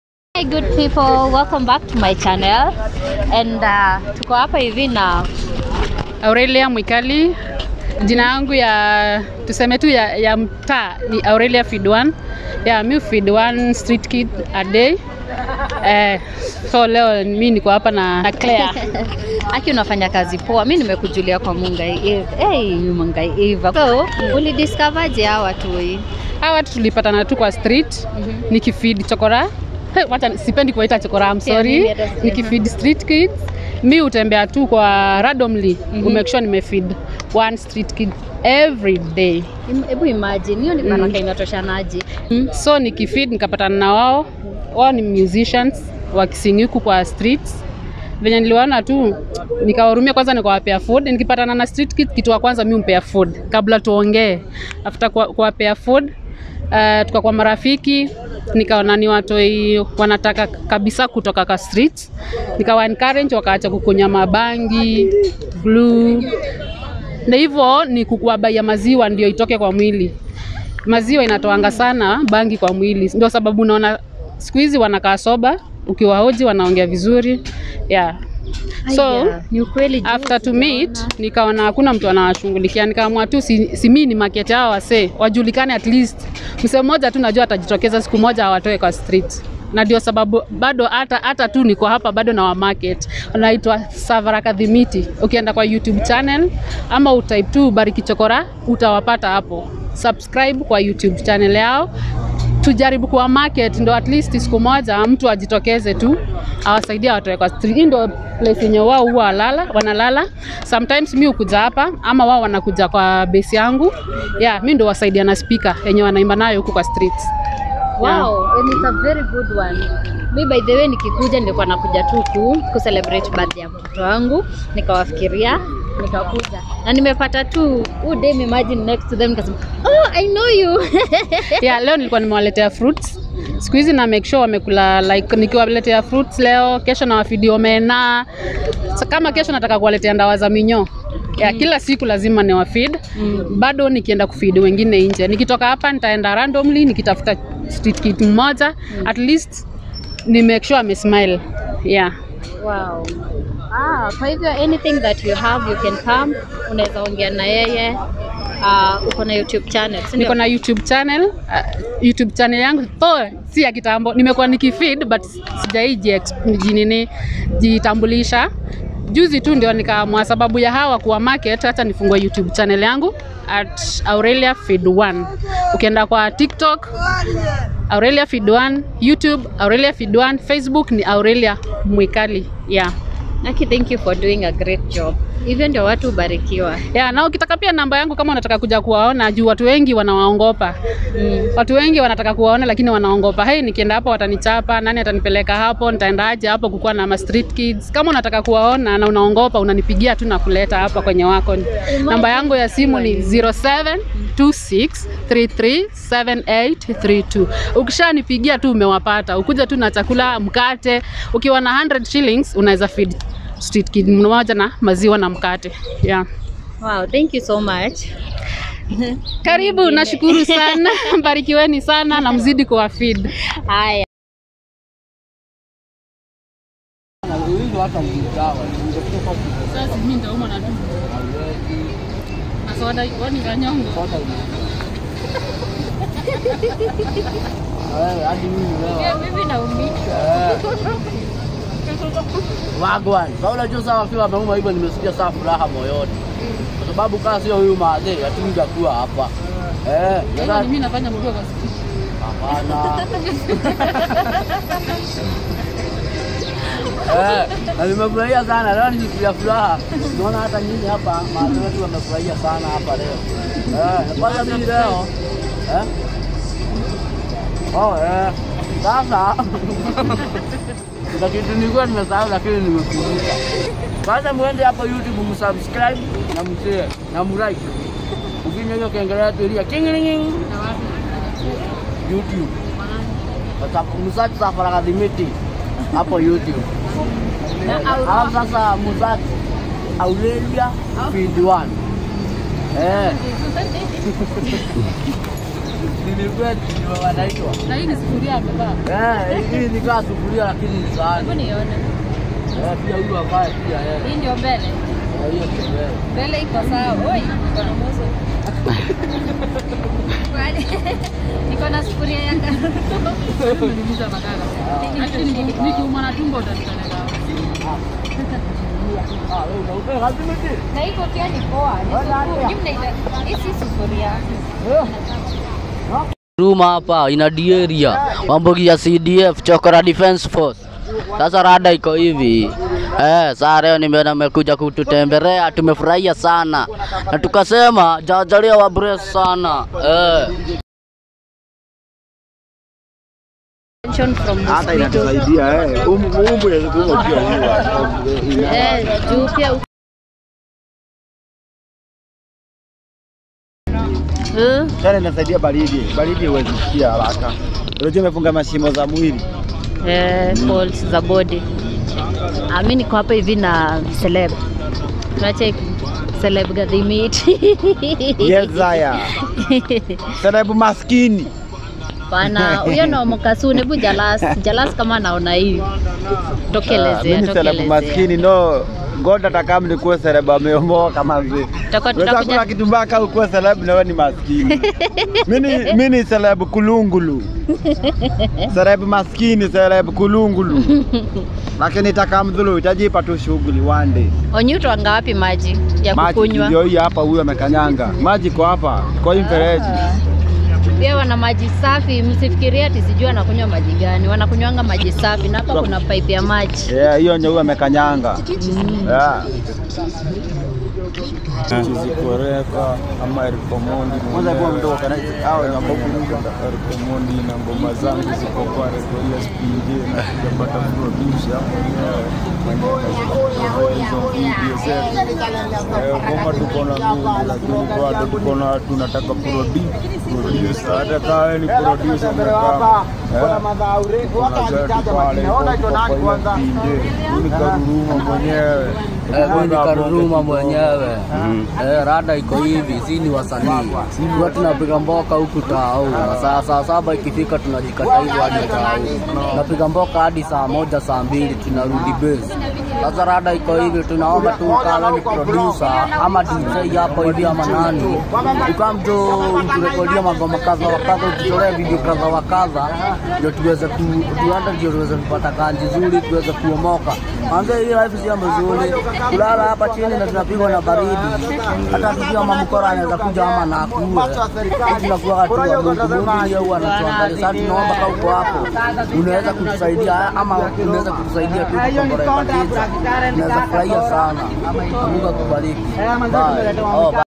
Creating gud moments with/safara gathimiti/karurumo/eric omondi/aureliafeed 1
I created gud moments with safara gathimitis gang, thy r fun to hang out with u guys should pay them a visit💖